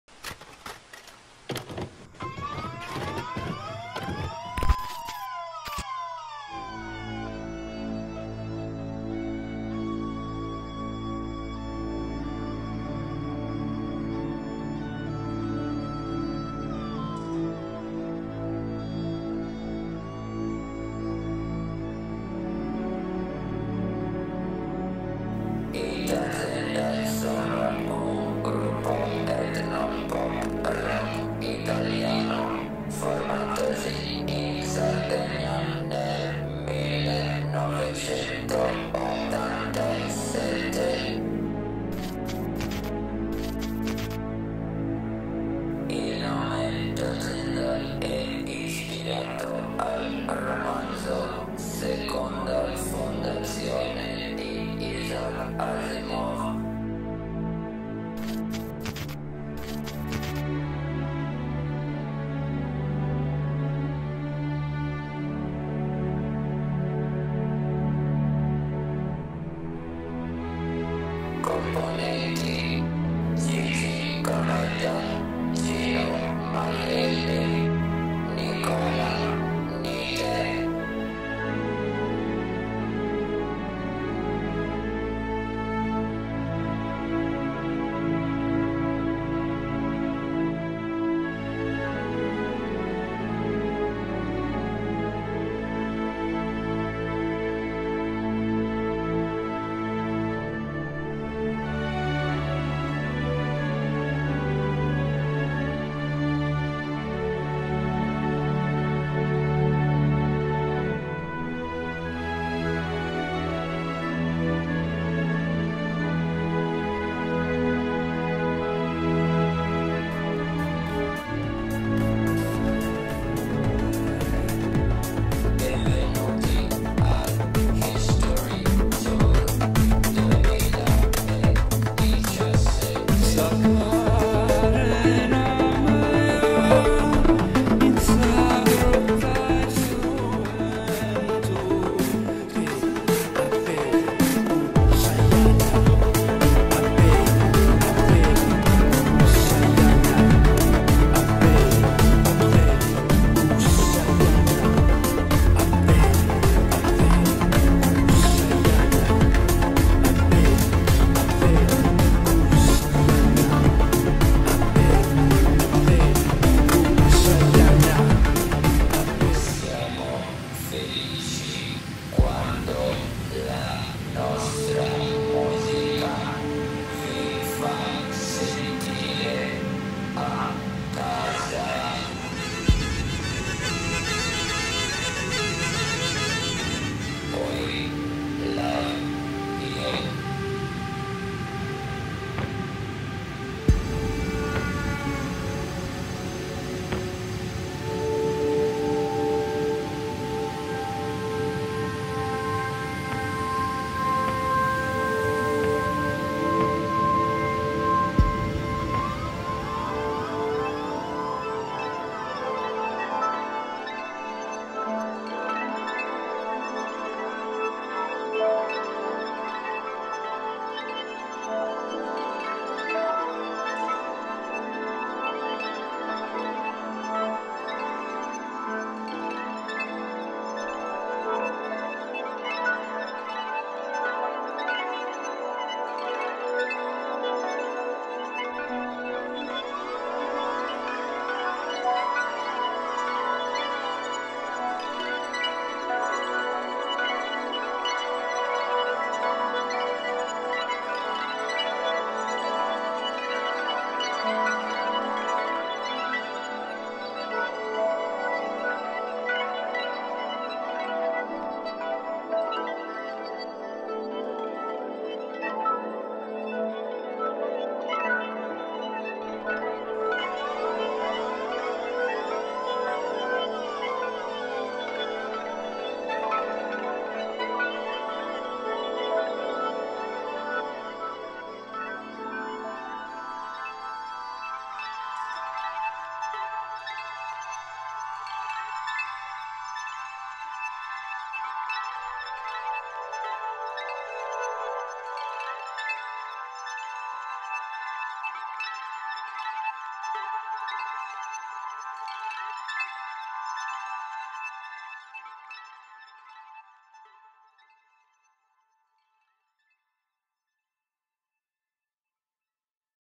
Tazenda History Tour 2017 • Intro Video Show
Video introduttivo per la serie di show "Tazenda History Tour 2017"
Realizzazione Renato Cubo • Soggetto Gino Marielli

Ogni immagine e ogni traccia audio è dei rispettivi proprietari